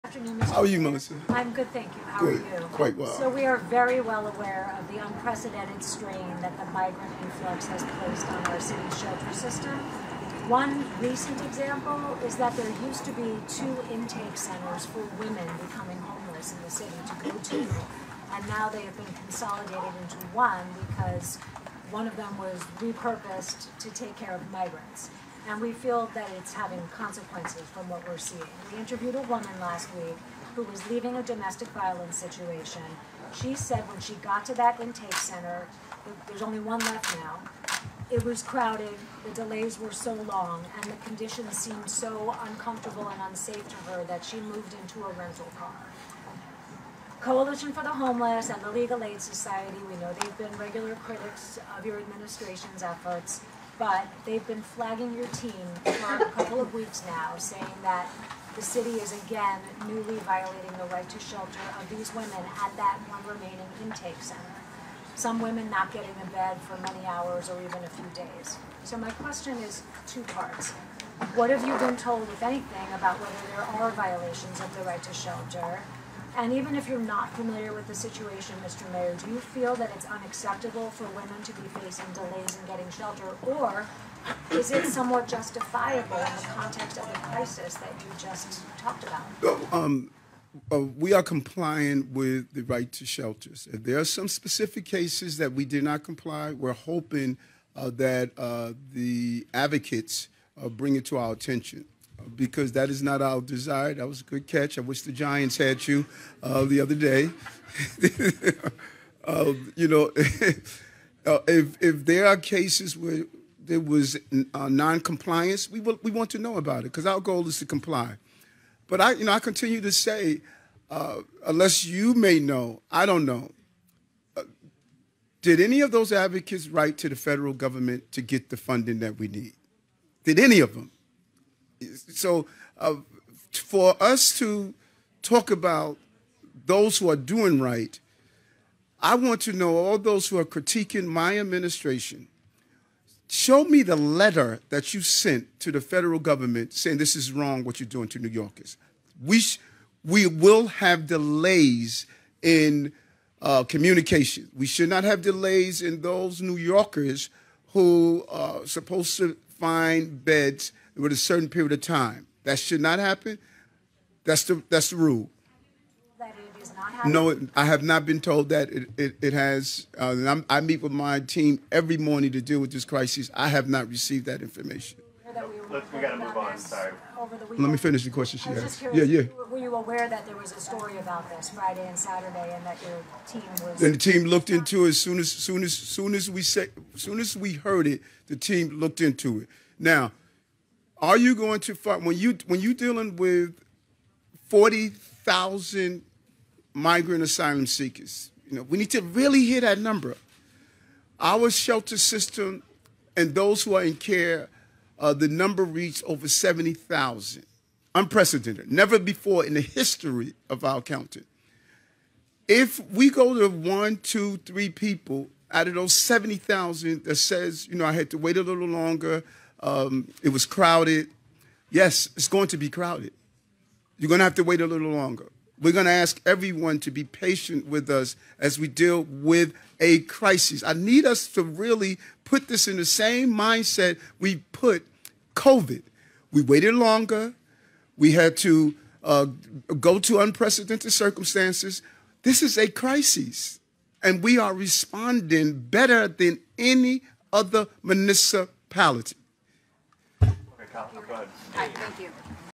Eric Adams Asked About Migrants Putting Strain On Shelter System
At a press conference on Monday, Mayor Eric Adams (D-NY) was asked about the strain that migrants have placed on New York.